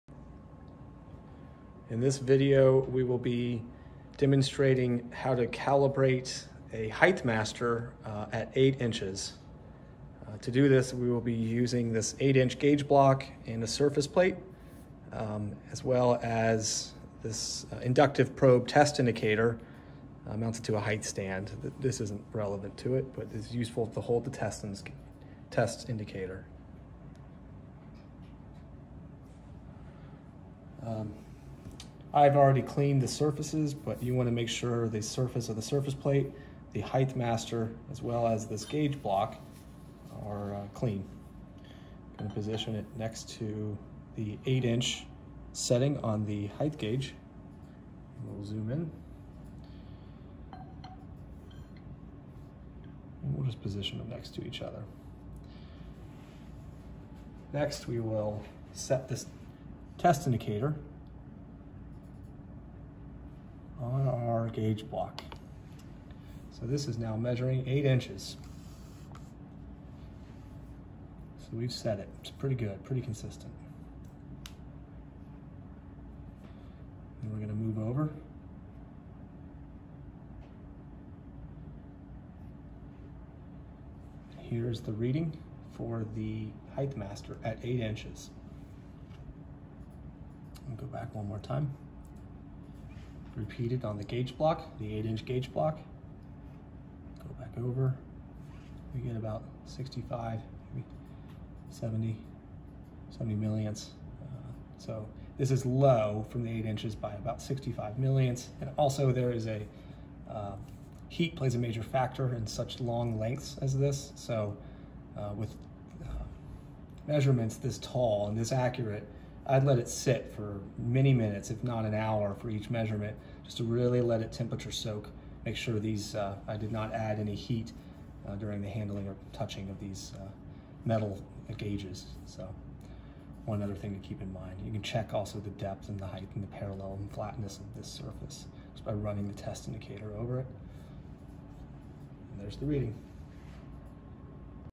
08 - Height Master Calibration - Audit 2021
This is a demo of how to calibrate a thread wire set for our 17025 audit.